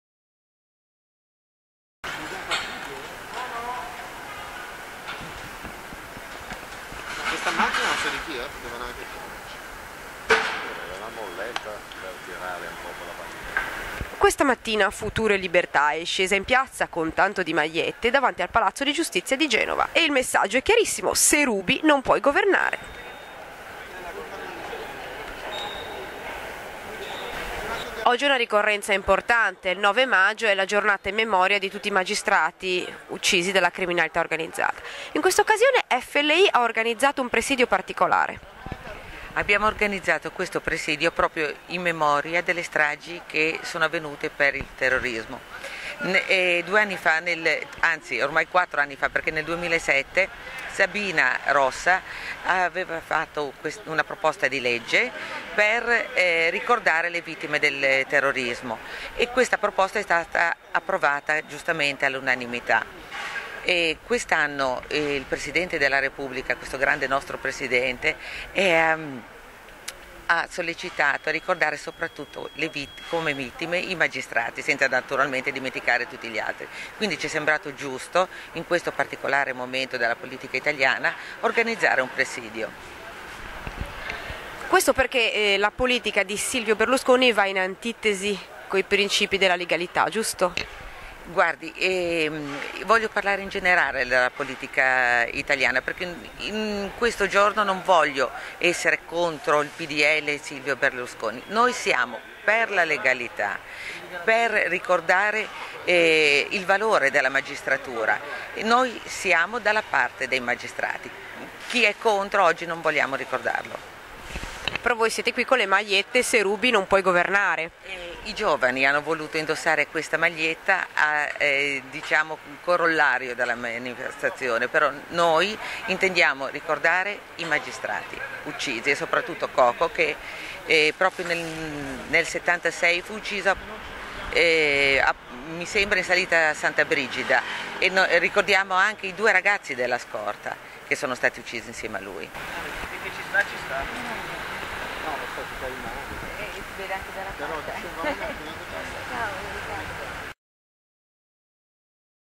FLI - Futuro e Liberta Genova - Manifestazione davanti al Tribunale - Se Ruby non puoi governare
FLI - Futuro e Liberta Genova - Manifestazione davanti al Tribunale - Se Ruby non puoi governare - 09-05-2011